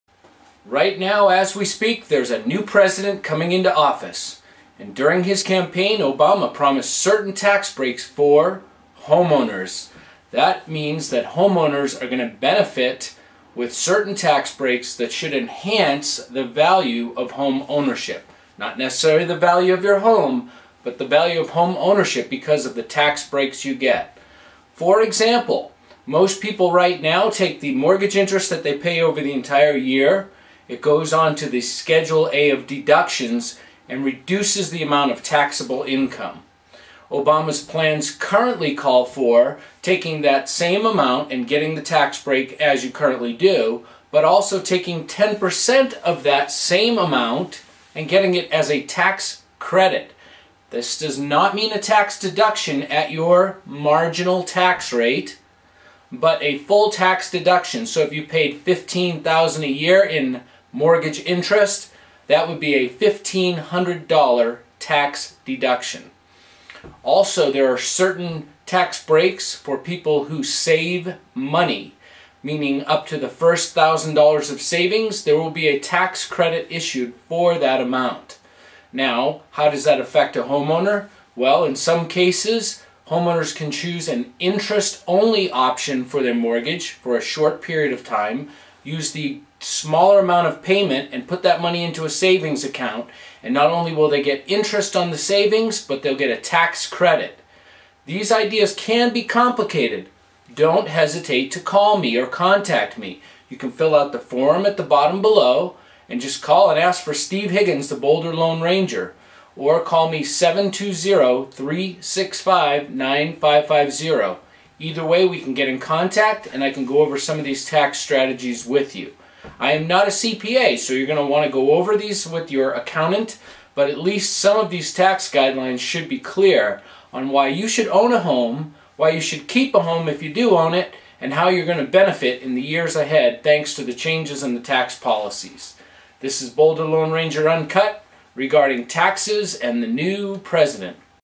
ObamaTax&You Mortgage Loan Tax Changes for New President
New President Means Home Mortgage Tax Changes - Know Them and Why You Should Own Real Estate!